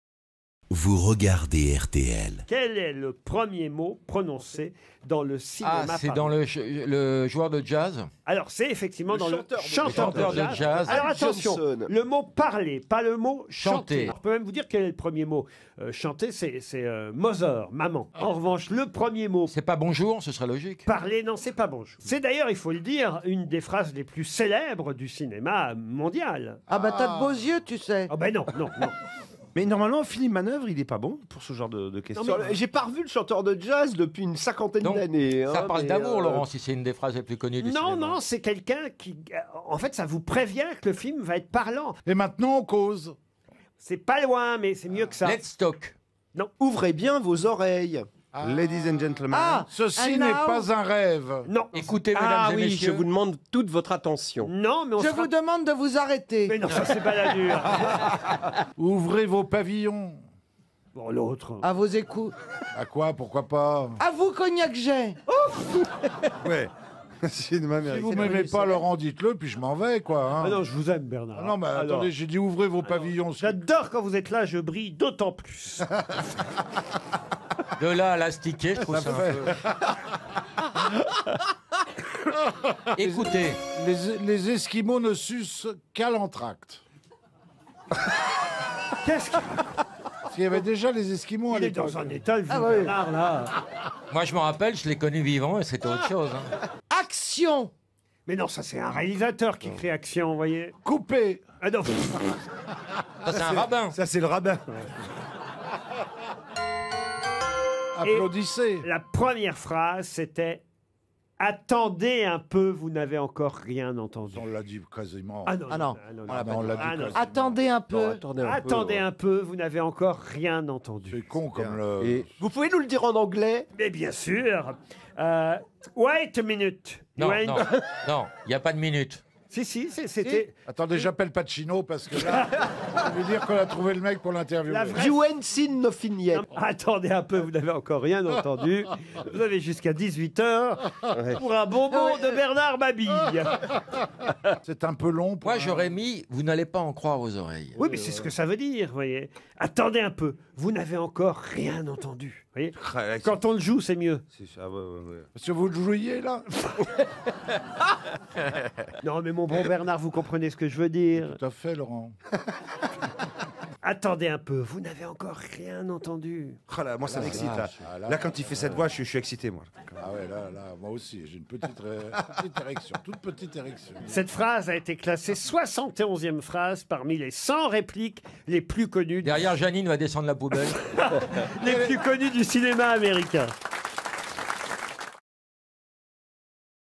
Le Premier Film parlant de l'Histoire du Cinéma
Laurent Ruquier demande à ses invités de trouver la première phrase prononcée dans un film. Une nouvelle occasion pour l'animateur de parler Anglais !